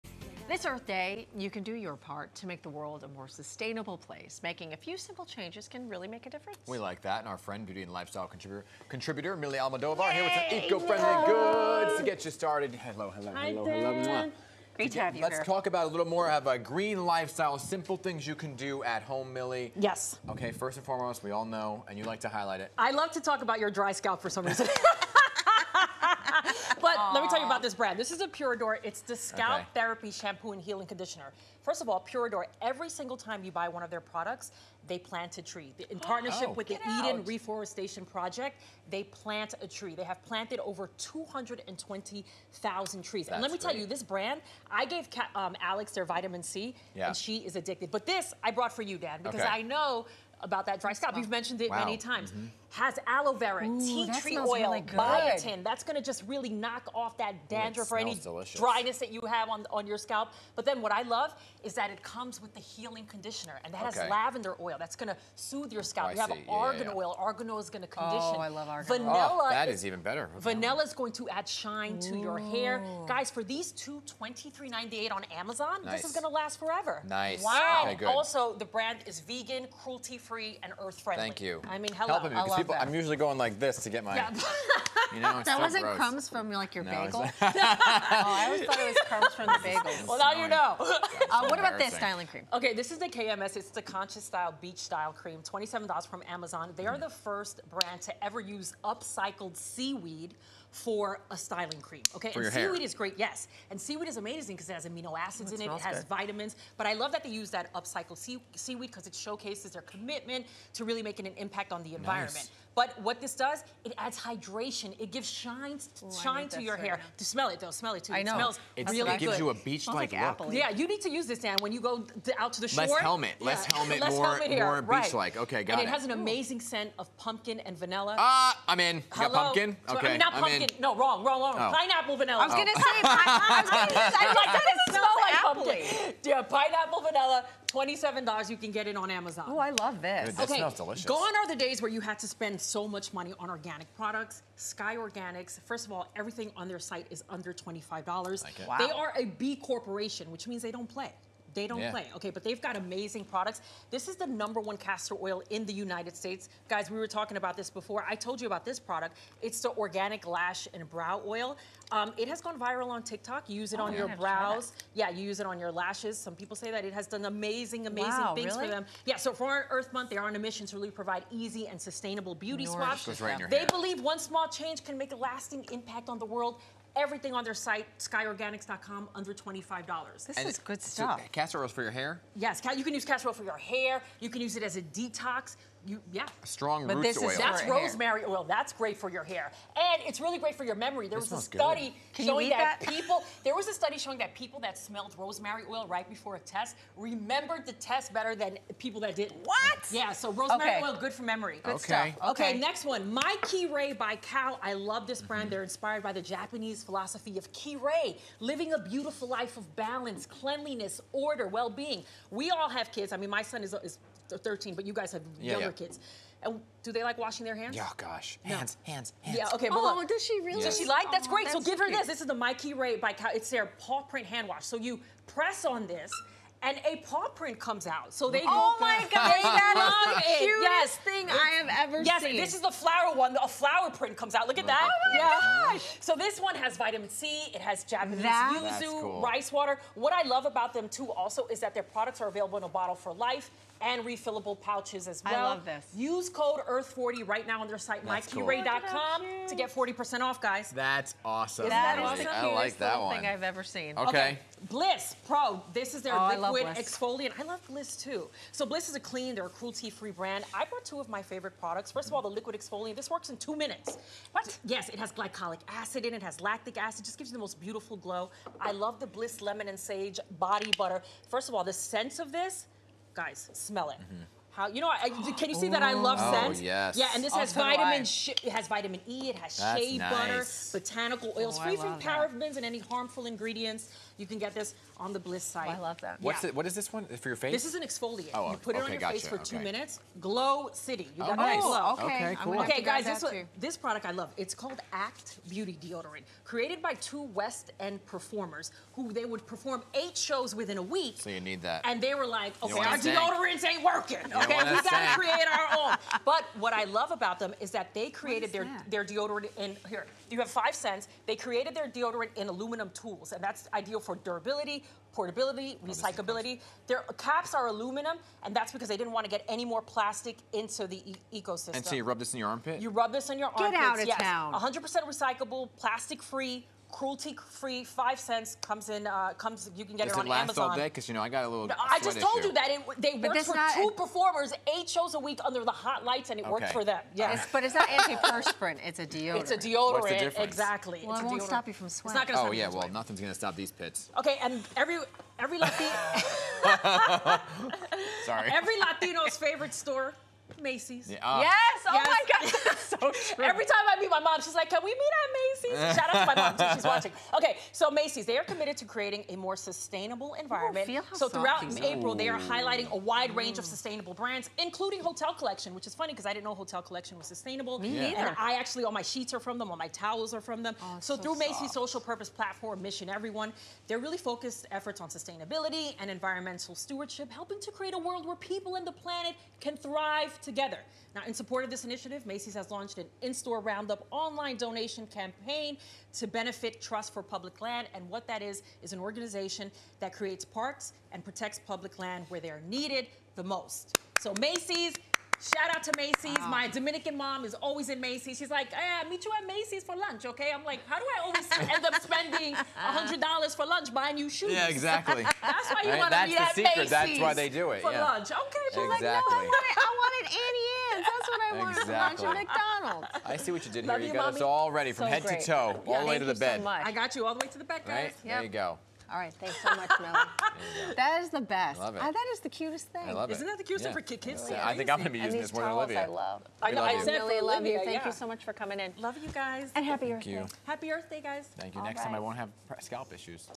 Sustainable goods for Earth Day
You can do your part this Earth Day to make the world a more sustainable place.

Beauty and lifestyle contributor, Milly Almodovar, has some eco-friendly goods to help you get started.